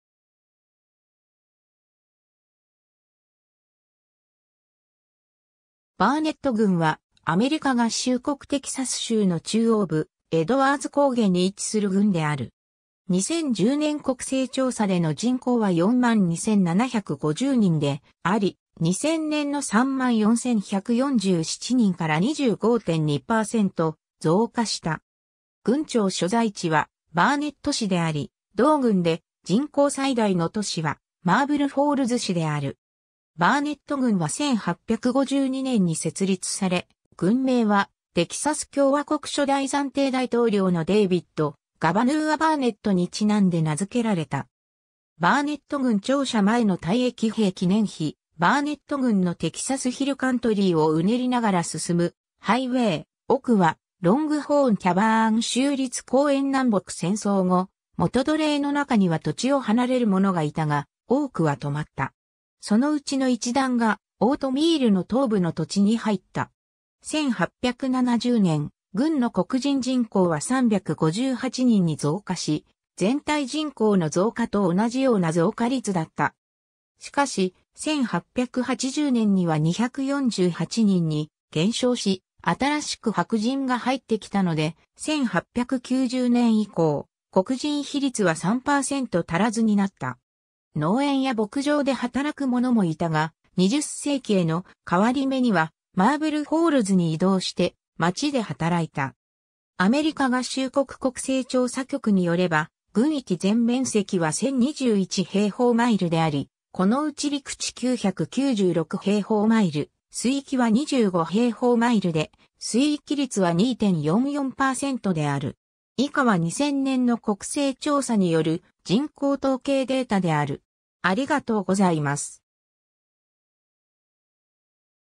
バーネット郡 (テキサス州)
バーネット郡（英: Burnet County）は、アメリカ合衆国テキサス州の中央部、エドワーズ高原に位置する郡である。
2010年国勢調査での人口は42,750人であり、2000年の34,147人から25.2％増加した。
郡庁所在地はバーネット市（人口5,987人）であり、同郡で人口最大の都市はマーブルフォールズ市（人口6,077人）である。
バーネット郡は1852年に設立され、郡名はテキサス共和国初代暫定大統領のデイビッド・ガバヌーア・バーネットに因んで名付けられた。
 バーネット郡庁舎前の退役兵記念碑 バーネット郡のテキサス・ヒルカントリーをうねりながら進むハイウェイ、奥はロングホーン・キャバーン州立公園 南北戦争後、元奴隷の中には土地を離れる者がいたが、多くは留まった。
そのうちの一団がオートミールの東部の土地に入った。
1870年、郡の黒人人口は358人に増加し、全体人口の増加と同じような増加率だった。
しかし、1880年には248人に減少し、新しく白人が入ってきたので、1890年以降、黒人比率は3％足らずになった。
農園や牧場で働く者もいたが、20世紀への変わり目にはマーブルフォールズに移動して町で働いた。
 アメリカ合衆国国勢調査局に拠れば、郡域全面積は1,021平方マイル (2,644.4 km2)であり、このうち陸地996平方マイル (2,579.6 km2)、水域は25平方マイル (64.7 km2)で水域率は2.44％である。